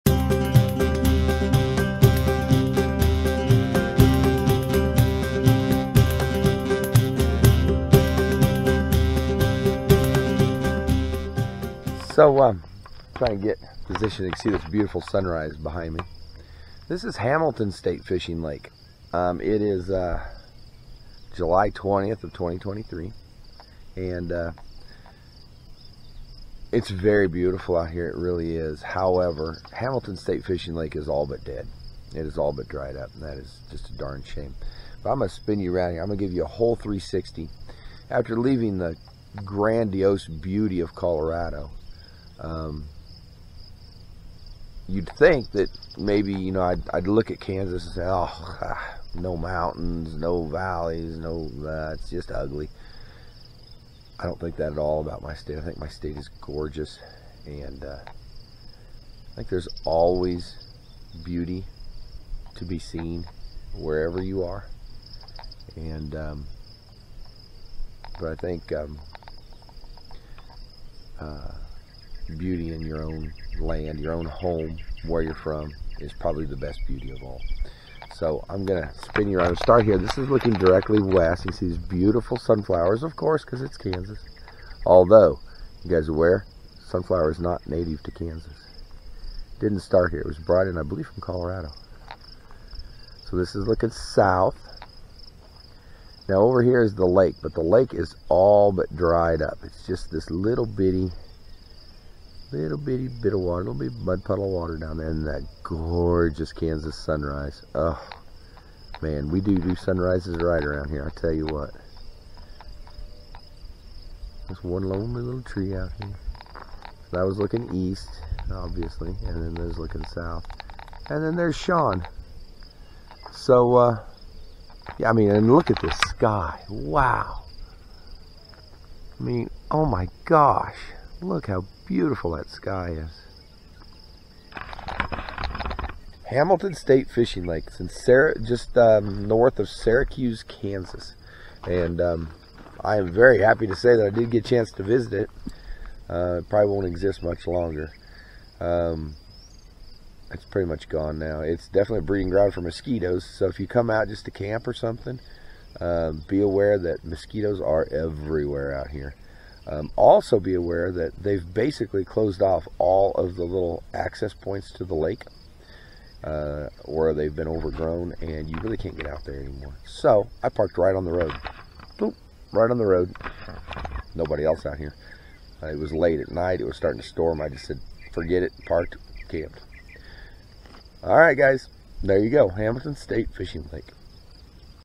TFF - Hamilton State Fishing Lake
Hamilton State Fishing Lake, Syracuse, KS.  This is EXTREME western Kansas folks.  Get out there before it's gone.